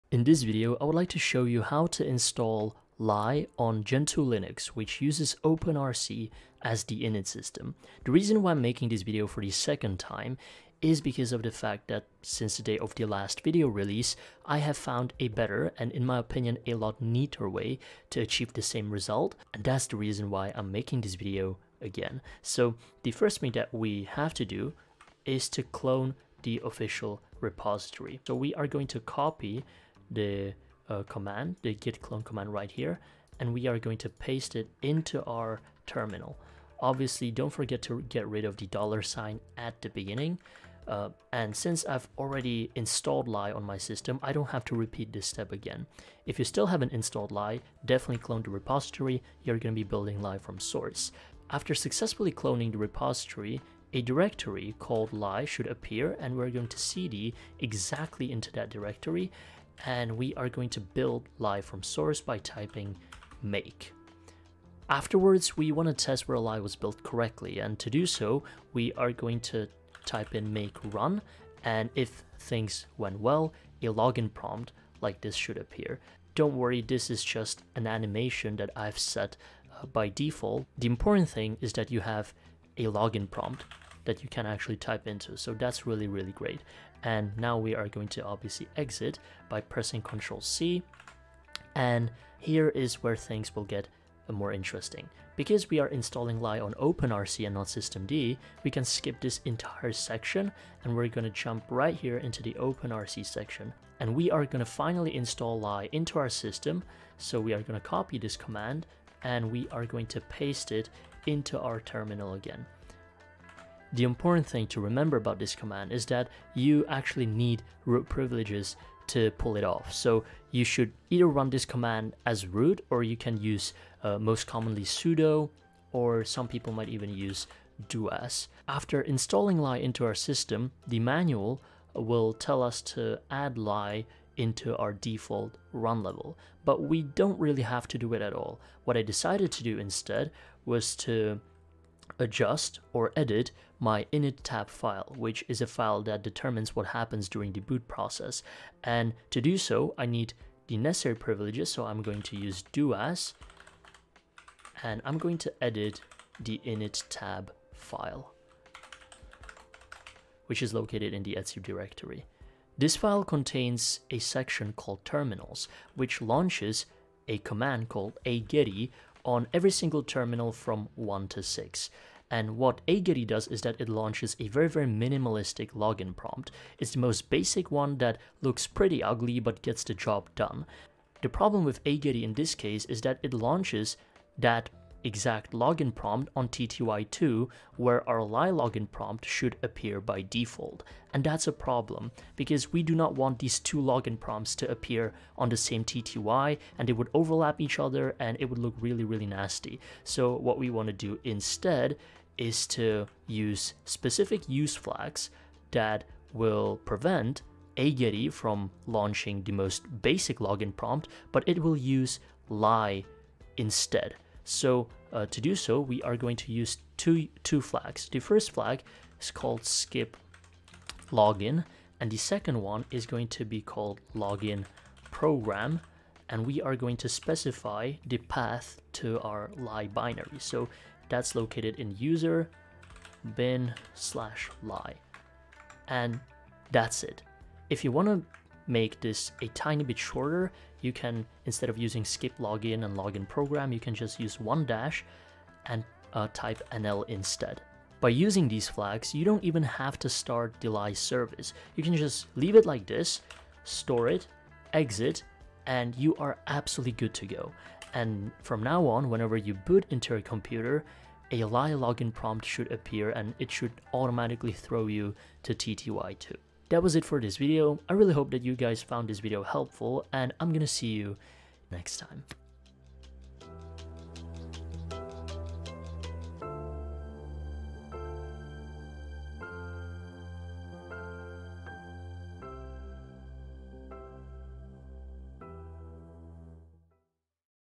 How to Install & Configure Ly on Gentoo Linux/OpenRC - Login Manager | SA1G0N
In this video, I would like to share how to configure Ly on Gentoo Linux, which uses OpenRC by default.

00:00 - Introduction
00:22 - Cloning
00:55 - Compiling
01:13 - Testing
01:44 - Installation
02:30 - /etc/inittab
04:16 - Outro